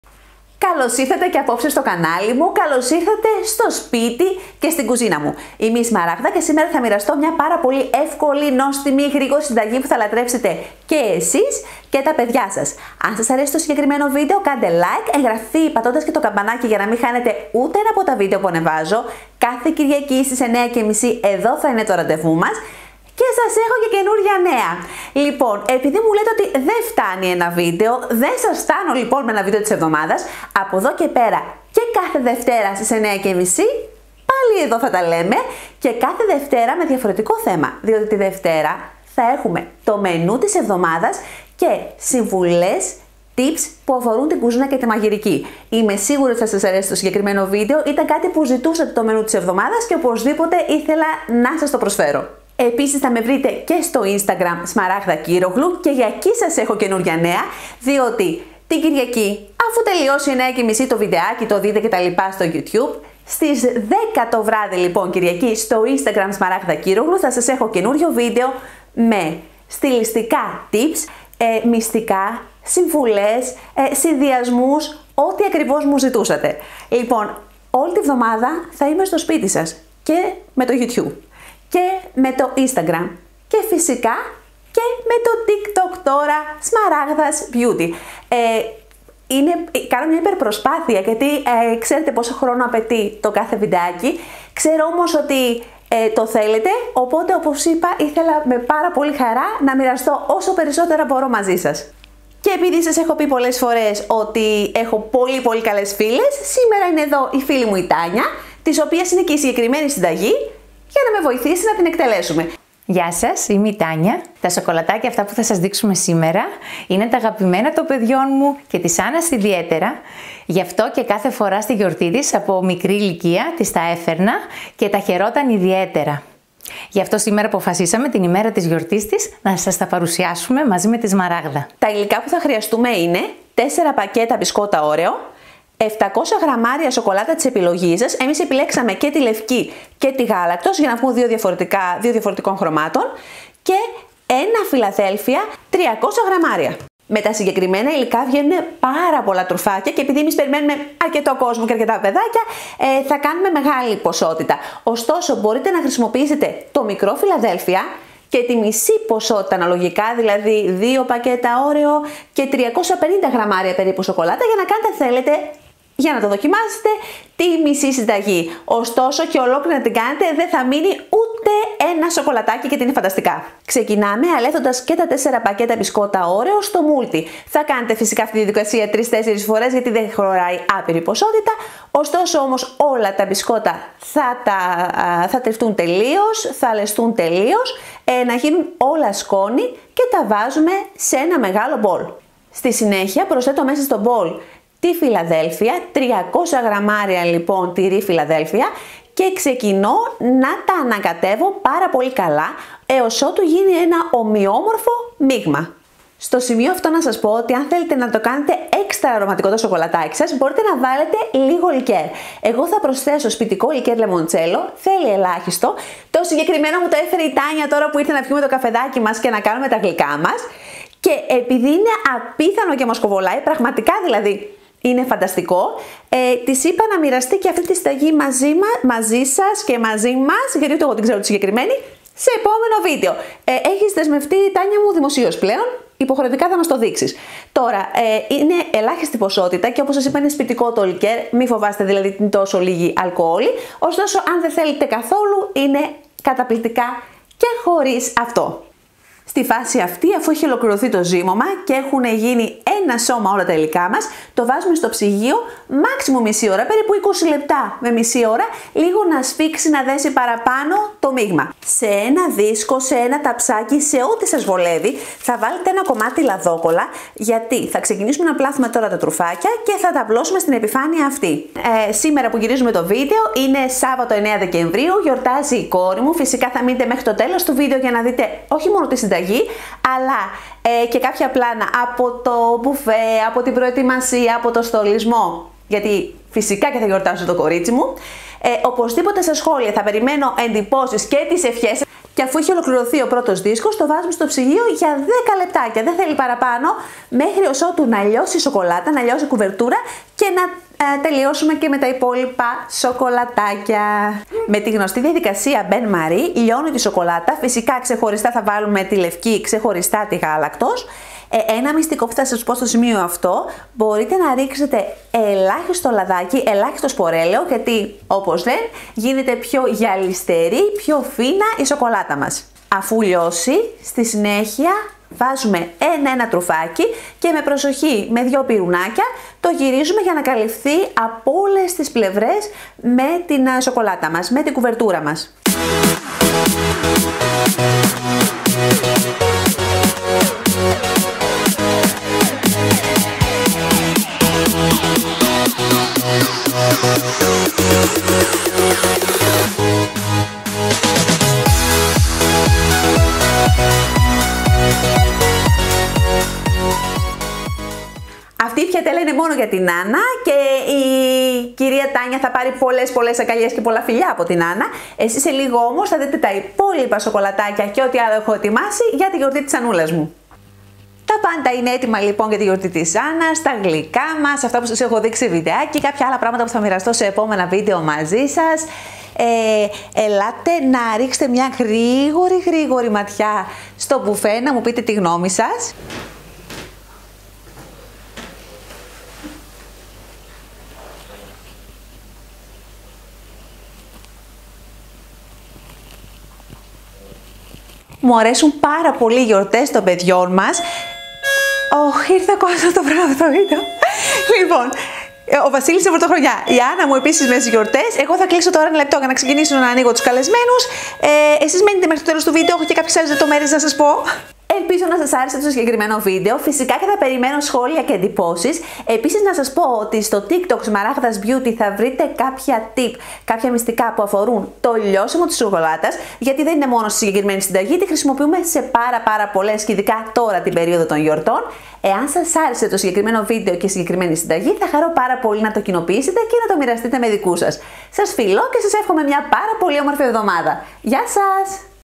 Τέλεια σοκολατάκια Όρεο με 3 υλικά & ο στολισμός για τη γιορτή της κόρης μου#cooking#cook#chocolate
Γεια σας!!! Το σημερινό βίντεο σας το παρουσιάζω μαζί με τη φίλη μου, Τάνια. Θα σας δείξουμε μια συνταγή αγαπημένη & πανεύκολη, που αρέσει φανατικά & σε μικρούς & σε μεγάλους!!! Σοκολατάκια Όρεο με μόνο τρία υλικά !!! Επίσης θα σας έχω πλάνα & από τη γιορτή της κόρης μου Άνναs💕💕💕
Θα χαρώ πολύ να μου στείλετε εντυπώσεις τόσο για τη συνταγή, όσο και για το στολισμό & το στήσιμο του μπουφέ !!!

Απαγορεύεται η αντιγραφή ολόκληρου ή μέρους αυτού του βίντεο χωρίς προσωπική γραπτή άδεια της δημιουργού του